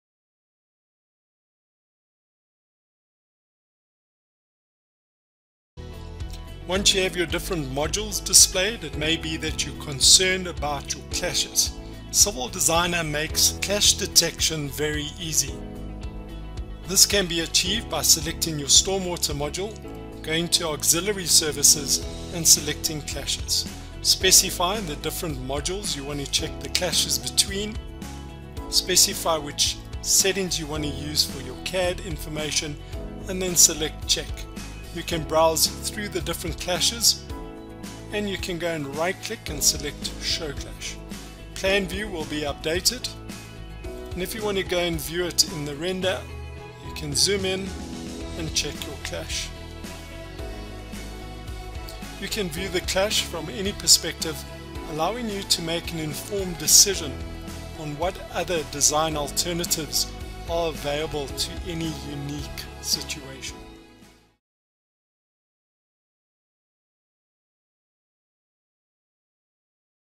Civil Designer software - detecting utility and services clashes.
Civil Designer makes the detection of utility and service clashes very easy. Avoiding potential clashes with existing services associated with your project in the early design phases ensures that you do not have to waste time handling proximity problems later on in the design process.